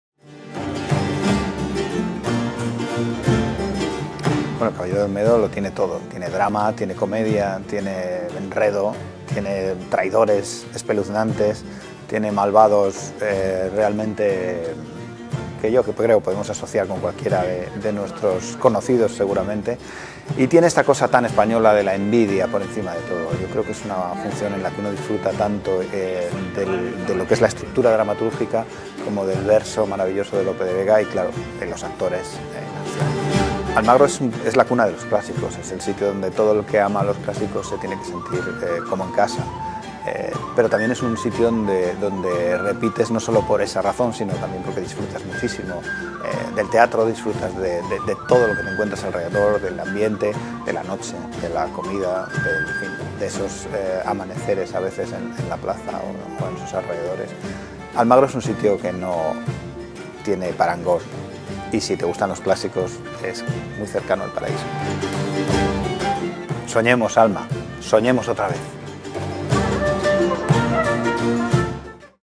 Eduardo Vasco sobre ‘El caballero de Olmedo’ y el Festival
“El caballero de Olmedo tiene de todo. Tiene drama, comedia, enredo, traidores, malvados…es una función en la que uno disfruta mucho de la estructura dramatúrgica, del verso maravilloso de Lope de Vega, de los actores…”

Eduardo Vasco, director de la obra, os cuenta por qué no debéis perderos ‘El caballero de Olmedo’ y el Festival de Almagro

El caballero de Olmedo -AUREA
26-28 julio 20.00h
Noviembre Teatro